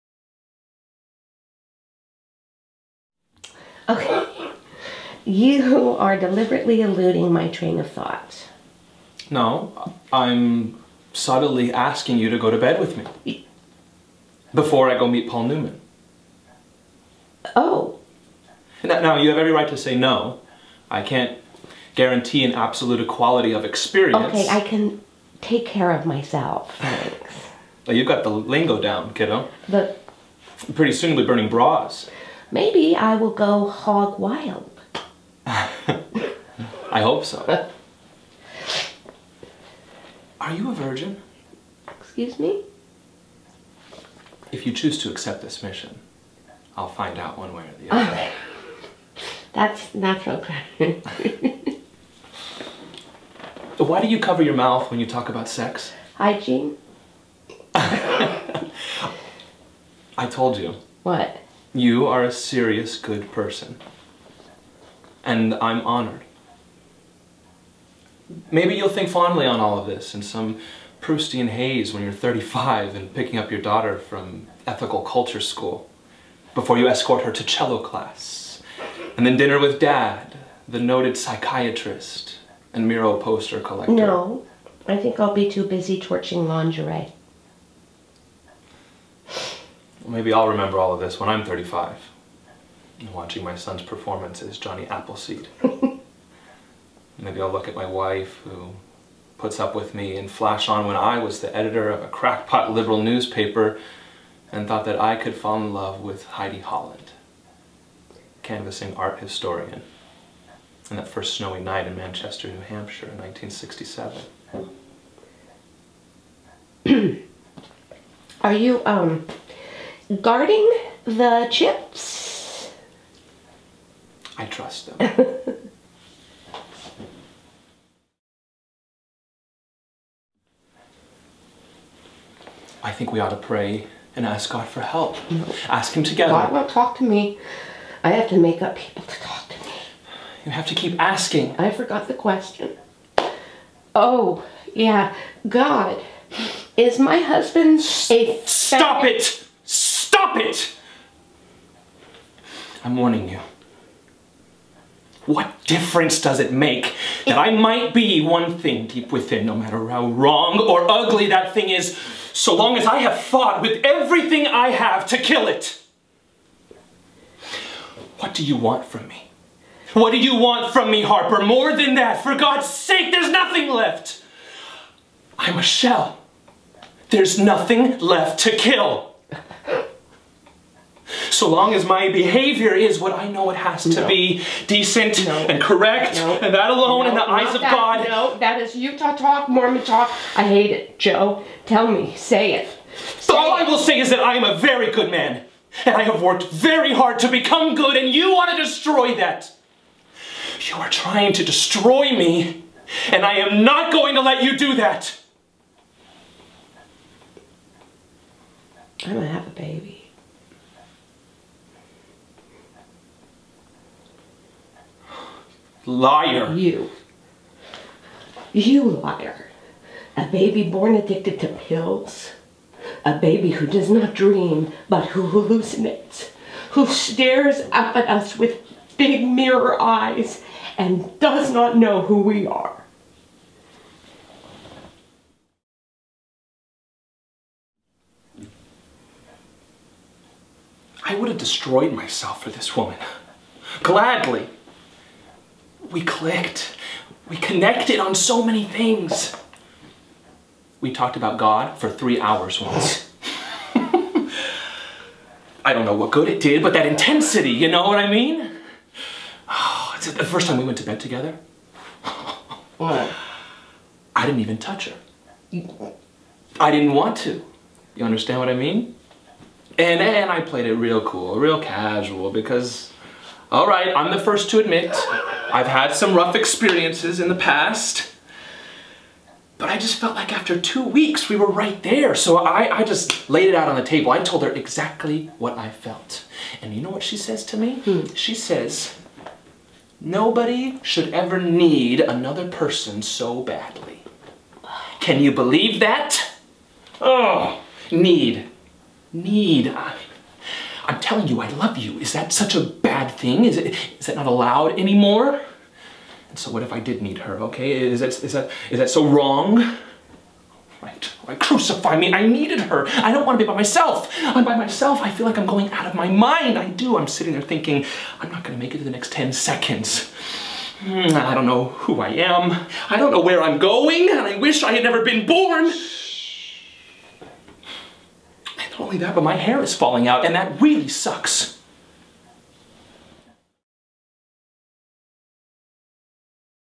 Jacob Pressley -- Acting Reel full
NYC based actor, Jacob Pressley performs scenes from The Heidi Chronicles, Angels in America and Boys Life.

0:00 The Heidi Chronicles by Wendy Wasserstein -- role of Scoop
2:09 Angels in America by Tony Kushner -- role of Joe
3:57 Boys Life by Howard Korder -- role of Phil

Produced at The Tape Room | NYC